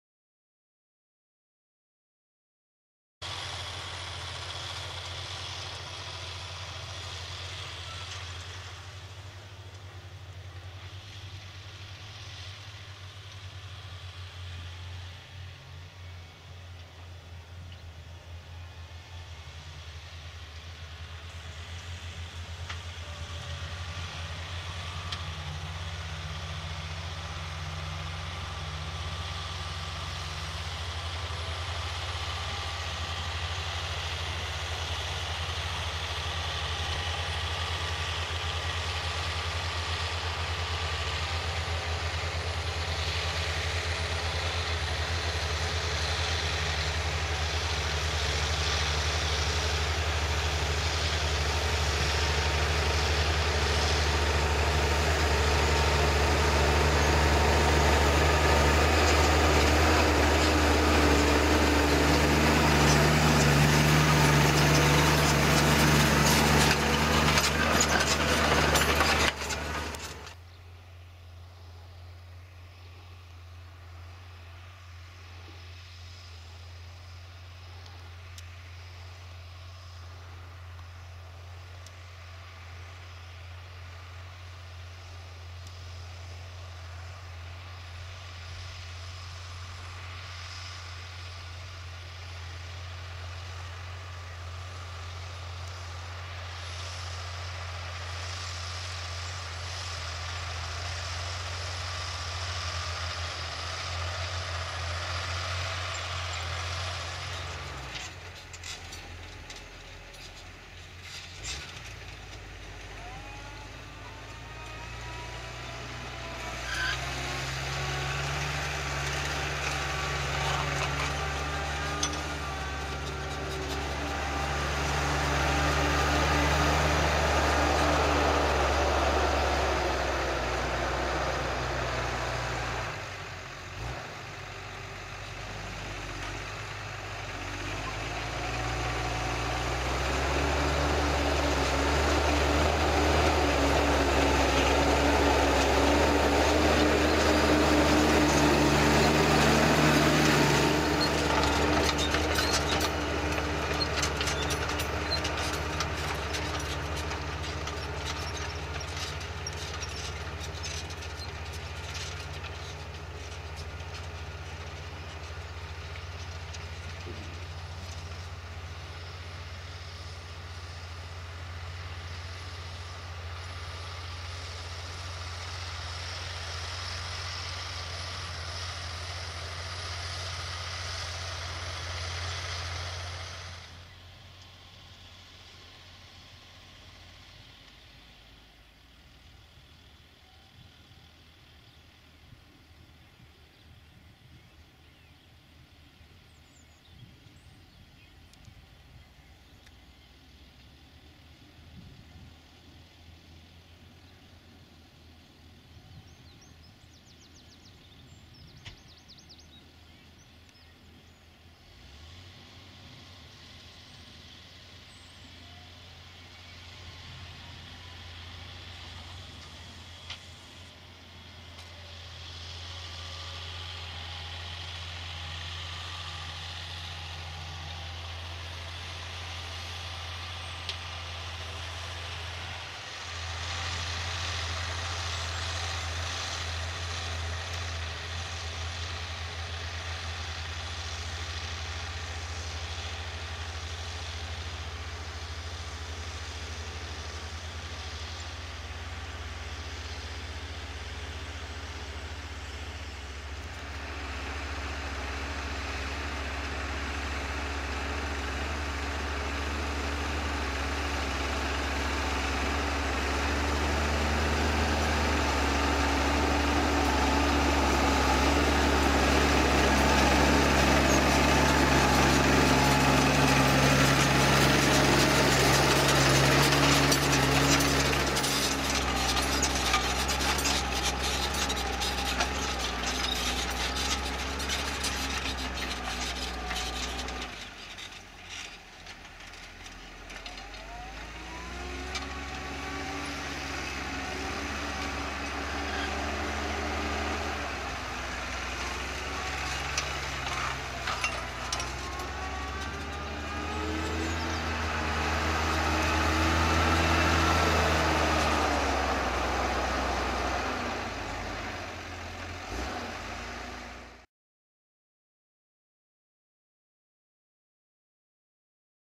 Ploughing a VERY Rough Field - and a Heavy Discing as Well.
Here we have a New Holland T6050 with Kverneland plough and a Massey-Ferguson 7480 with heavy discs.  They are working down a field that was recently purchased and is being restored to productive use. Maize next year!  Rob Manor is doing the ploughing and the discing is "own work".
Filmed in 2012 in Carmarthenshire, Wales, UK.